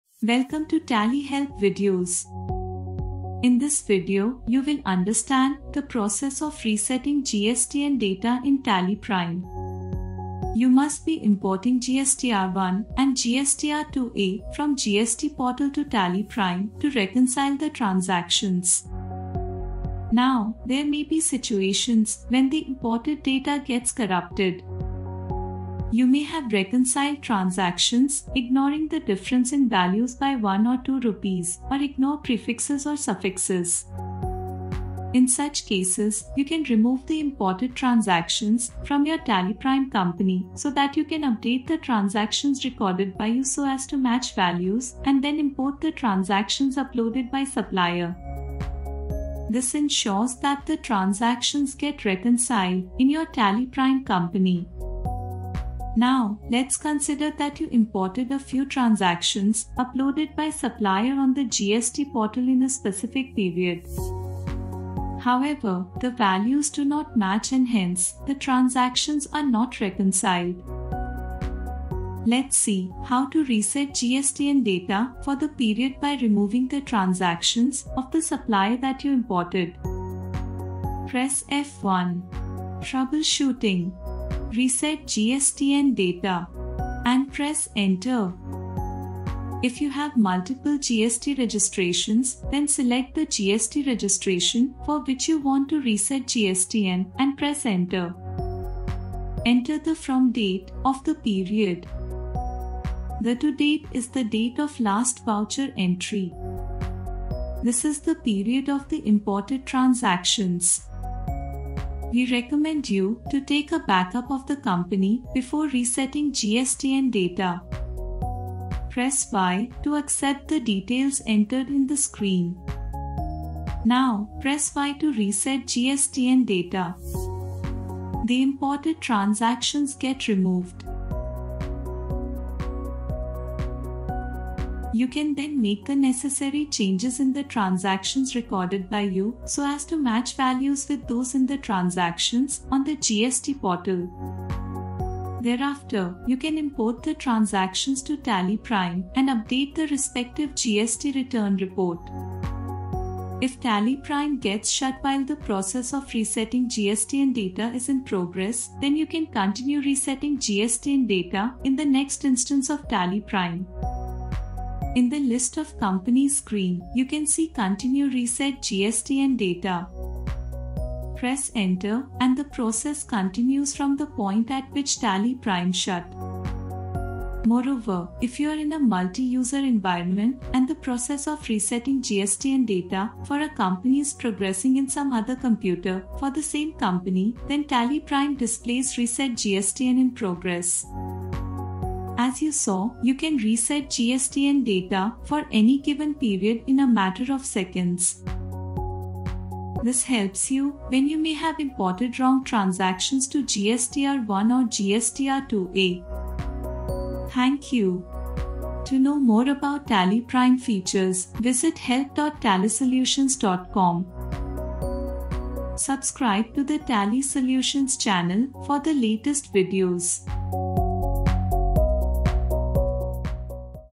How to Reset GSTN Data in TallyPrime | TallyHelp
Applicable to Release 3.0 and later

You import the transactions from GSTR-1 and GSTR-2A from GST portal to TallyPrime. However, there may be situations in which the values do not match with the transactions recorded by you due to unknown reasons.

It may also be possible that the data downloaded from the portal got corrupted.

In such scenarios, you can reset GSTN data for the period by removing the imported transactions.

Resetting GSTN data helps you make the necessary changes so as to match values with the transactions in GSTR-1 and GSTR-2A that are uploaded on GST portal.

00:00 - Title and Introduction
00:20 - Why and when to reset GSTN data
01:12 - How to reset GSTN data